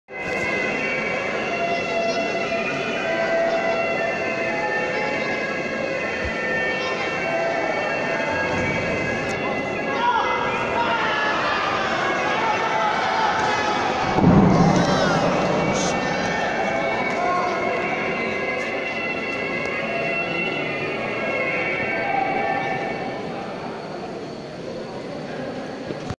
MUAY THAI PROFESSIONAL FIGHT SESSION1 IN CKB COMMERCE COLLEGE AUDITORIUM JORHAT
Deadly knock out in the match...